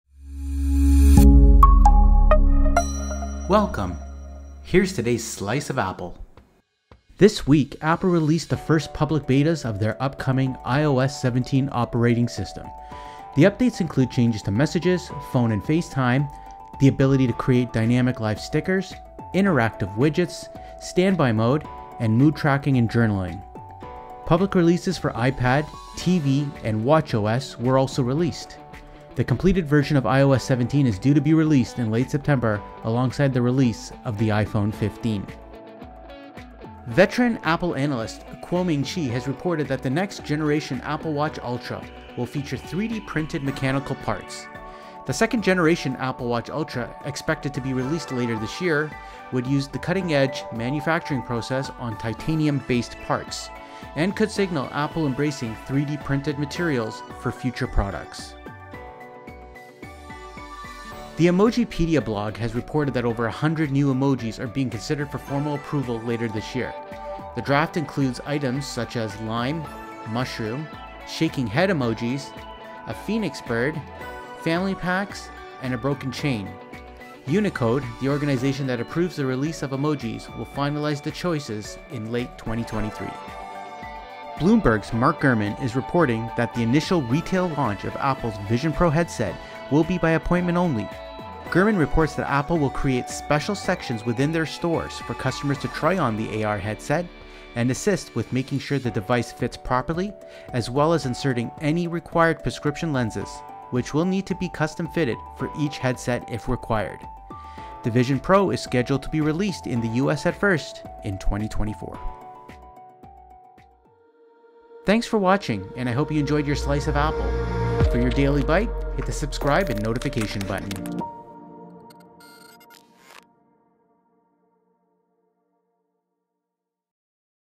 Apple News- iOS17 Public Beta, New Emoji
In today's Apple News:

0:00 iOS 17 Public Beta
0:35 3d Printing for Apple Watch Ultra
1:00 2023 Emoji Finalists
1:23 Pro Vision by Appointment